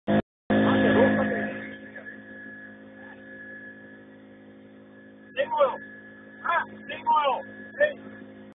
Intervento particolare condotto ieri nella Roggia Girosa a Bressanvido.
A seguito della fuoriuscita da un impianto di piscicultura  nella roggia di un notevole quantitativo di novellame di trota iridea si è dovuto procedere al loro recupero.